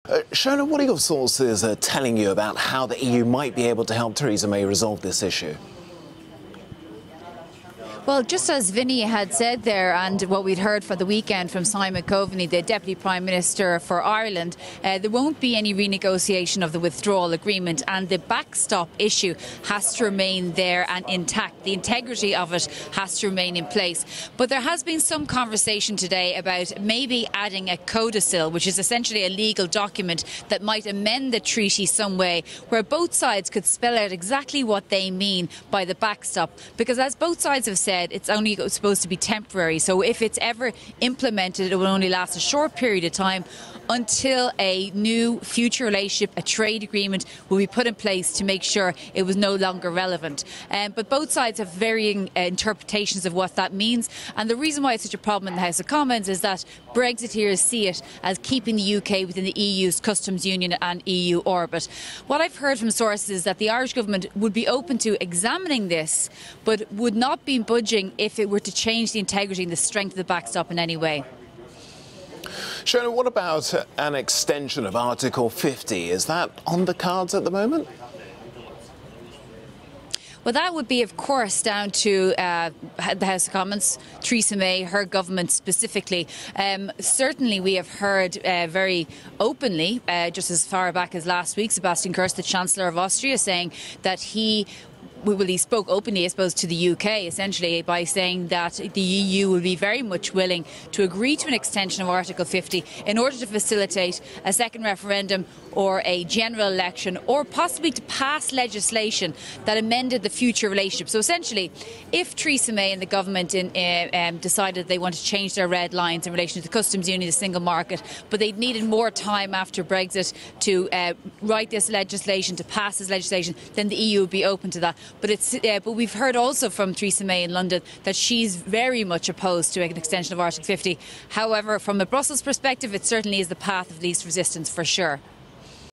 The Irish backstop is a major hurdle for Theresa May
The Irish backstop is a major hurdle for UK Prime Minister Theresa May, but will the EU be able to help May resolve this issue? Is an extension of Article 50 on the cards?

@ShonaMurray_ has more on this from Brussels.